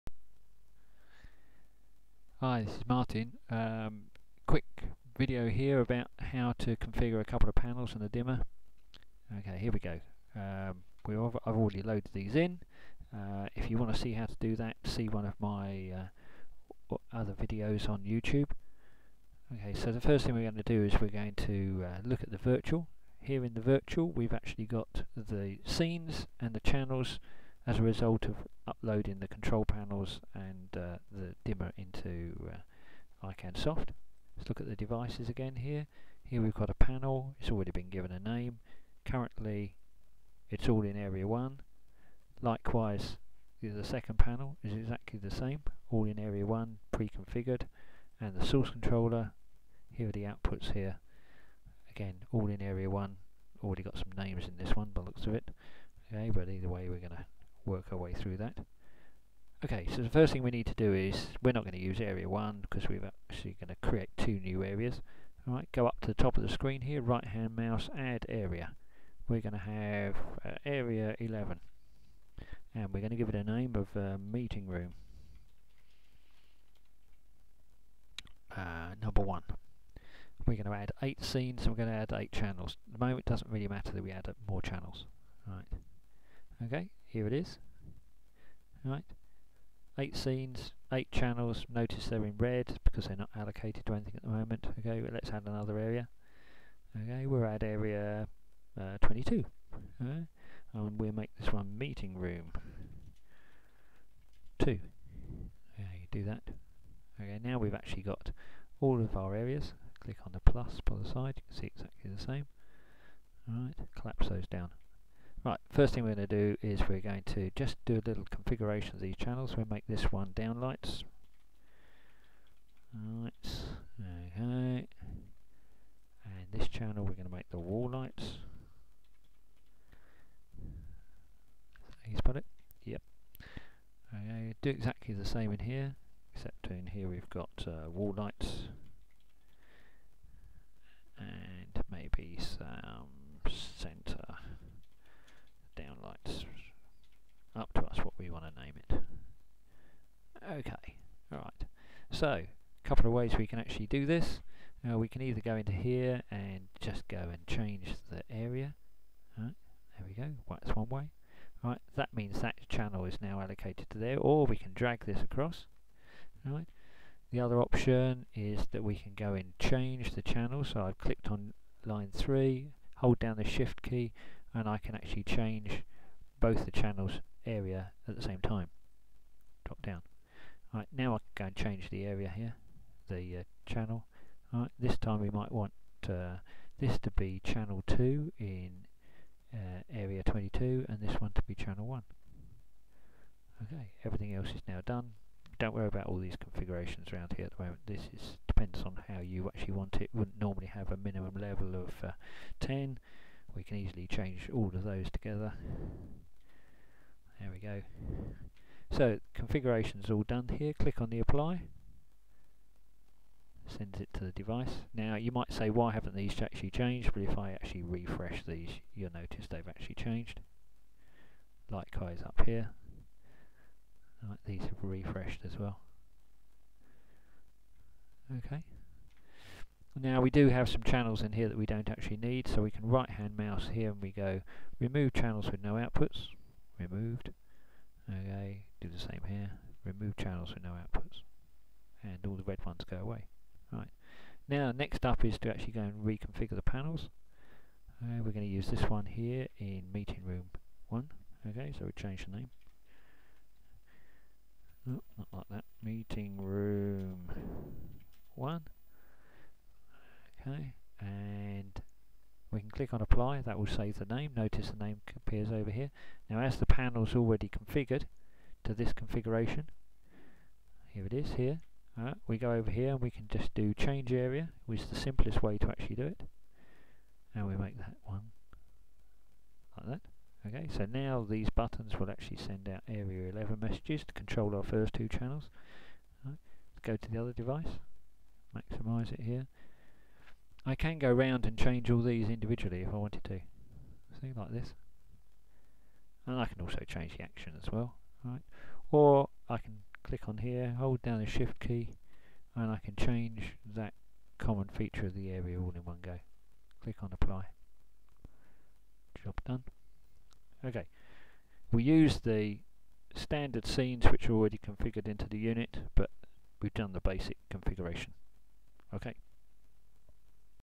Basic configuration of small iLight System
Shows you how to create areas and then configure two control panels and source controller. Best to see the video that shows you how to loacte devices on the network and load them into iCANsoft first.